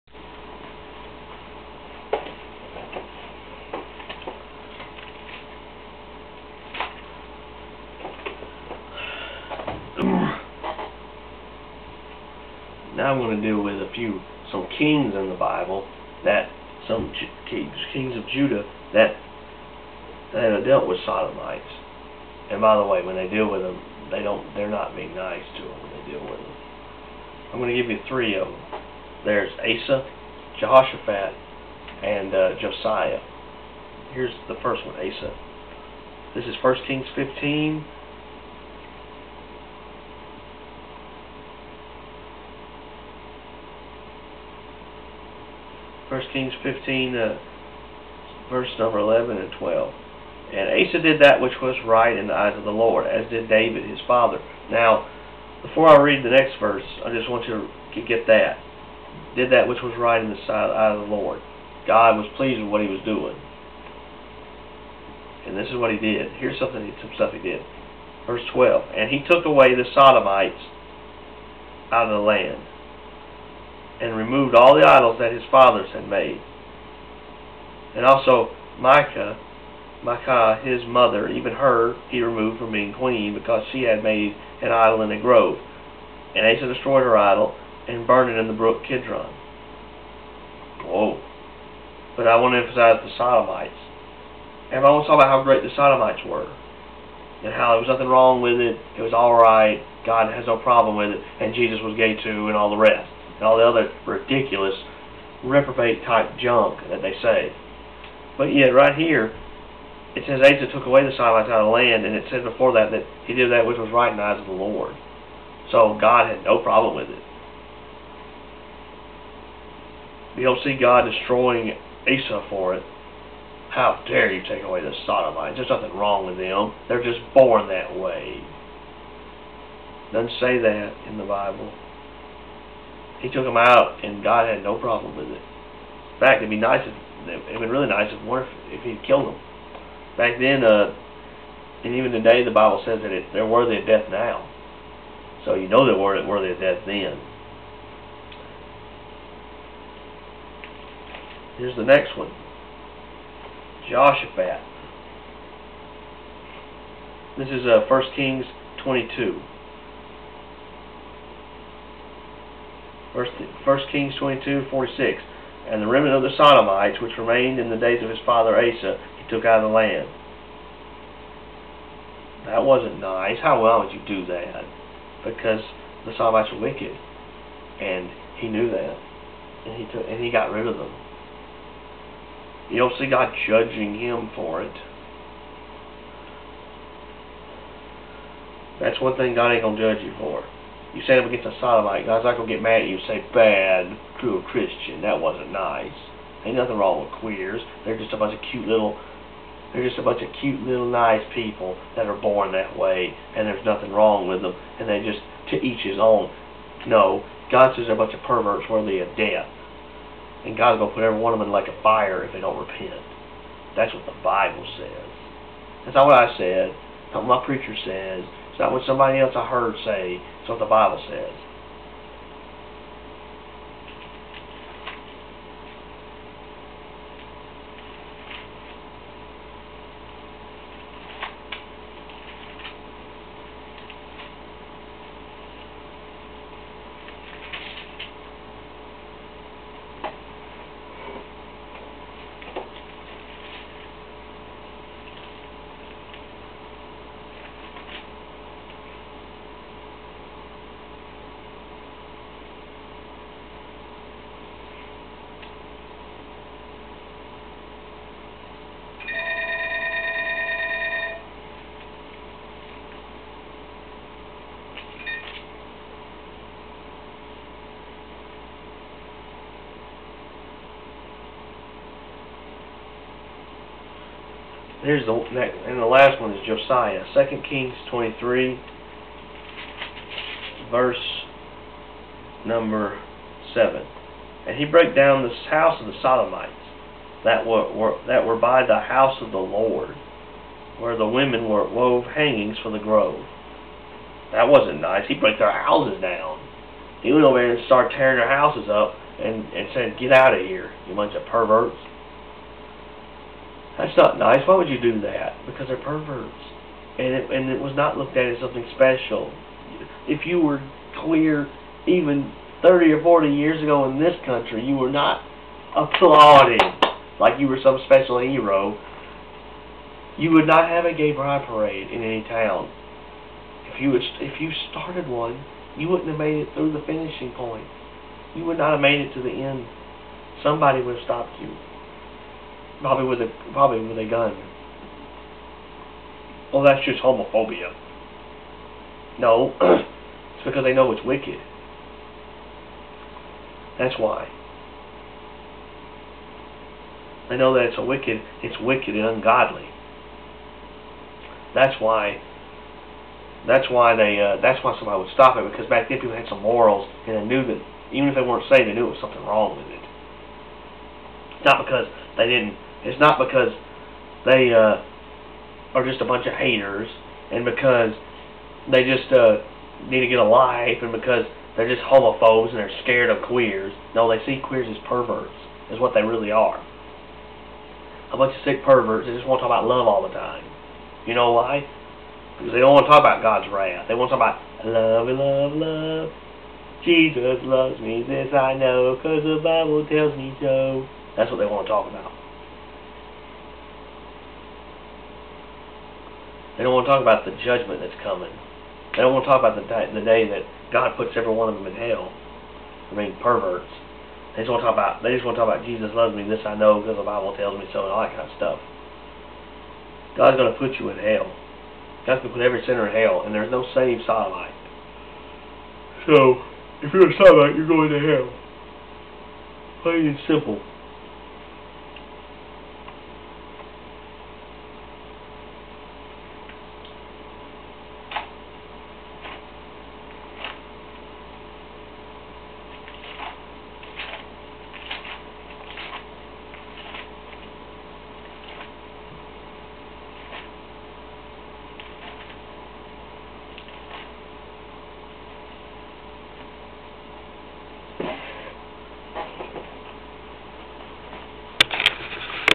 THE SIN OF HOMOSEXUALITY 4/7
THIS WILL SHOW WITHOUT A DOUBT THAT SODOMY AKA HOMOSEXUALITY IS SIN AND WICKED AND VILE ACCORDING TO THE BIBLE AND IT MAKES GOD O MIGHTY JESUS CHRIST SICK!